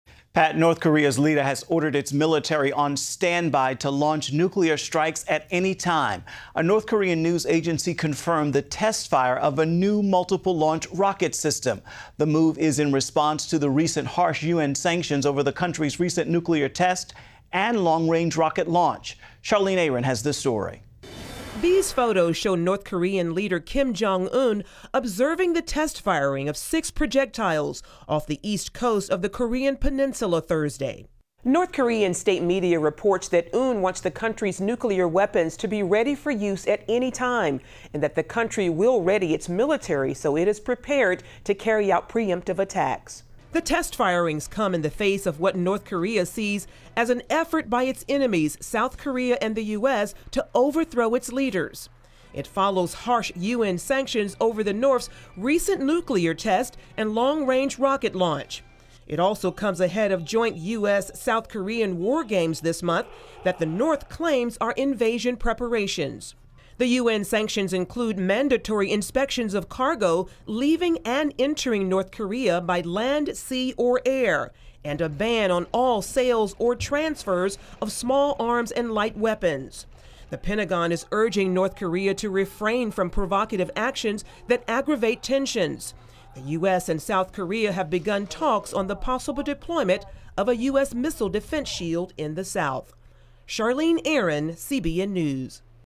North Korean Military 'On Standby' for Nuclear Strikes
North Korea put its military on standby for nuclear strikes that could come at any time, while its state-controlled news agency confirmed the test-firing of a new multiple-launch rocket system.